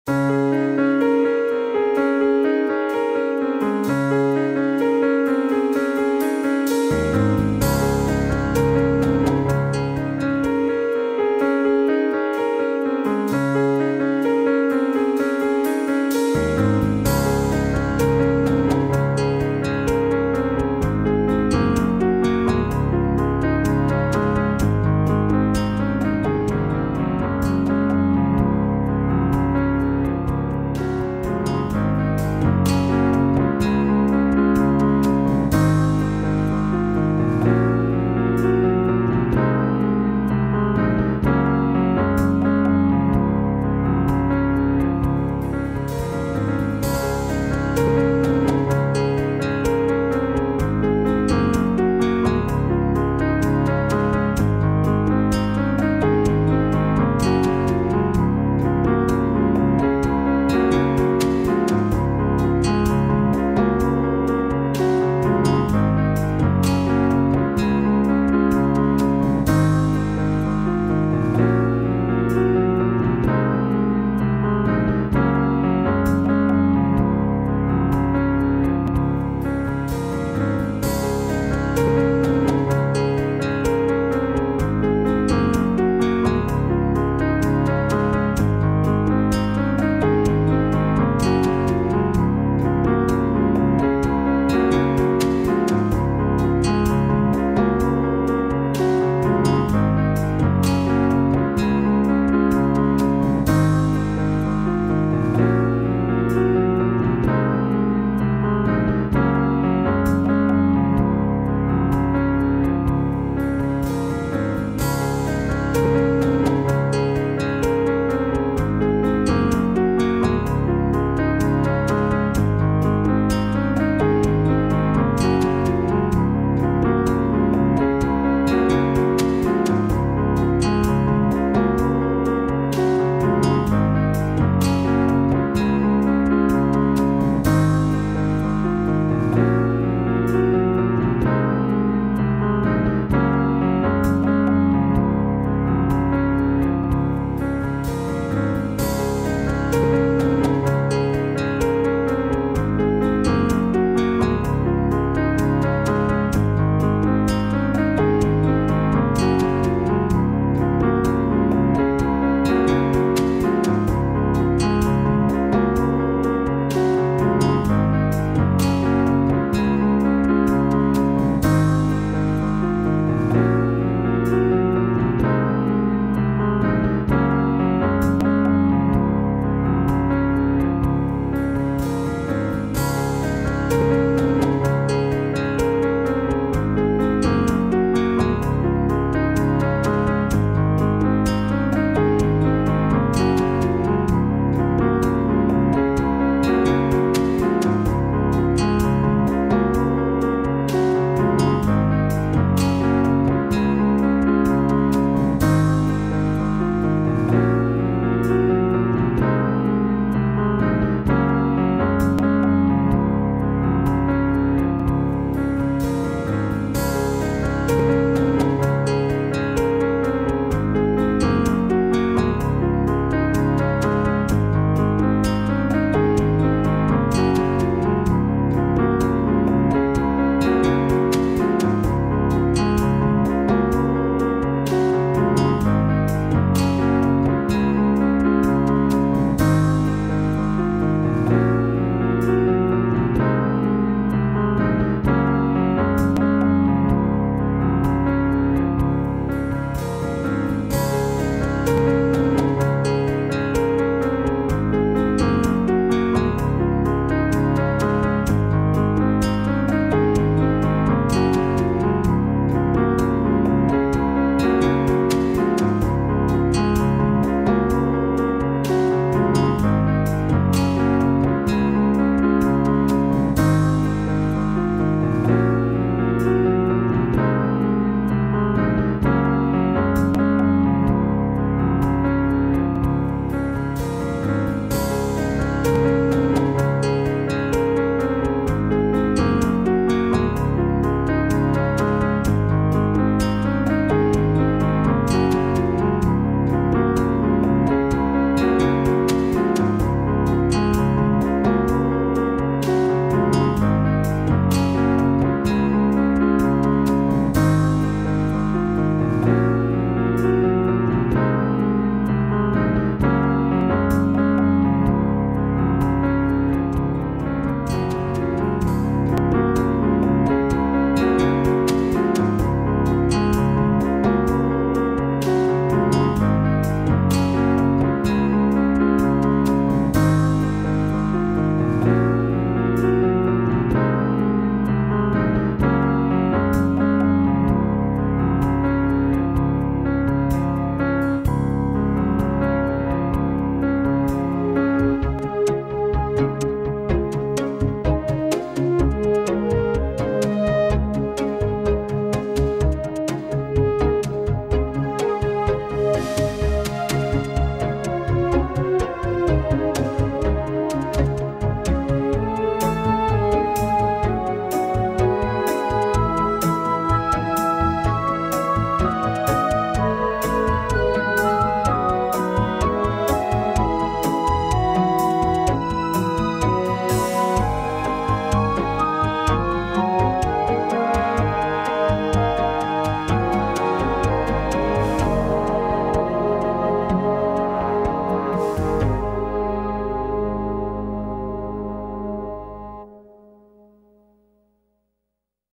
Speed CAS of the Dockery Family
Meet John & Mary Dockery! Residents of '1890 Newcrest'. Catch them in my Decades Let's Play as local townies!